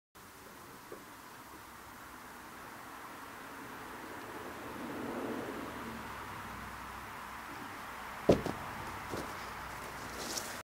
jump off deck
me jumping off my 10 foot deck